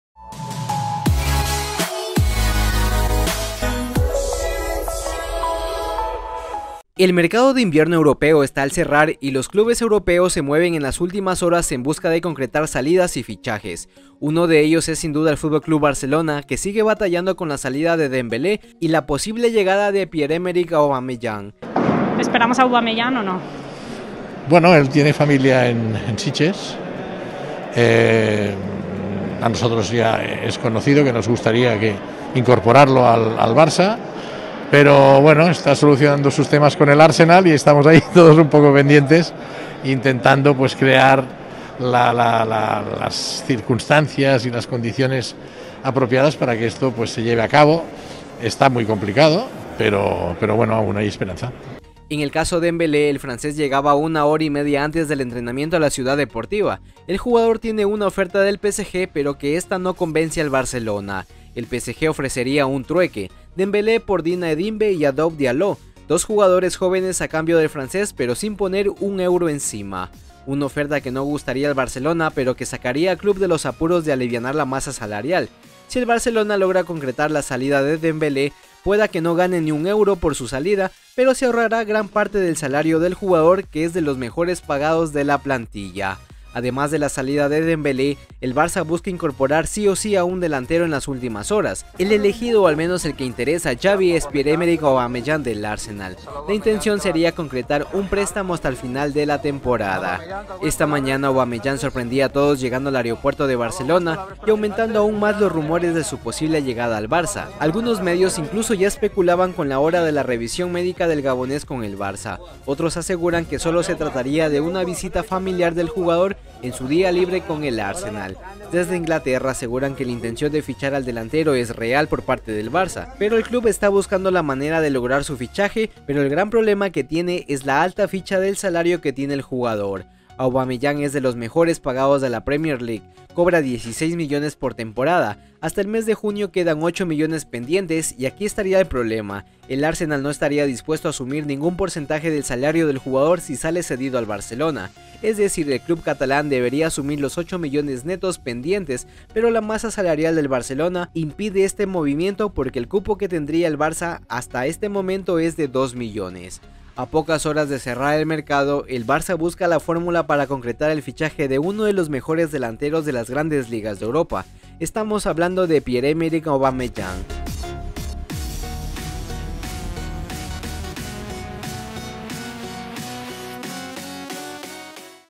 LAPORTA habla de AUBAMEYANG ¿Qué PASA con AUBAMEYANG, LLEGARÁ al FC BARCELONA?, DEMBELÉ SALIDAS
Joan Laporta ha asegurado este lunes por la tarde que el Barça apurará las últimas horas del mercado de enero. El presidente, que ha acudido a un acto en Barcelona, no ha escondido que todas las carpetas siguen abiertas encima de la mesa, sobre todo Aubameyang y Dembélé.

"Se está trabajando y se trabajará hasta el último momento. En todos los temas", ha asegurado Joan Laporta al ser preguntado por los dos jugadores que en estos momentos llenan la actualidad en clave culé. Por lo tanto, el presidente sigue muy pendiente del móvil en estas últimas horas que quedan para el cierre de la ventana de fichajes.

Una de las opciones que se baraja es la de Aubameyang que ha viajado a Barcelona. "Nos gustaría incorporarle al Barcelona. Estamos a la espera de que se solucione sus problemas con el Arsenal. Trabajamos entre todos para que se den las circunstancias apropiadas y podamos traerlo. Está complicado, pero tenemos esperanzas", dijo el presidente.

El Barcelona está siendo uno de los grandes protagonistas en este 31 de enero. Ya lo fue en el mercado de verano, cuando apuró hasta la medianoche todas las operaciones. Ahora el presidente ha confirmado que también estarán pendientes hasta las 00.00 horas de poder cerrar operaciones.

Laporta quiere que Xavi tenga el equipo más competitivo posible para esta recta final de temporada, donde los azulgranas se juegan la Europa League y, aunque está complicada, LaLiga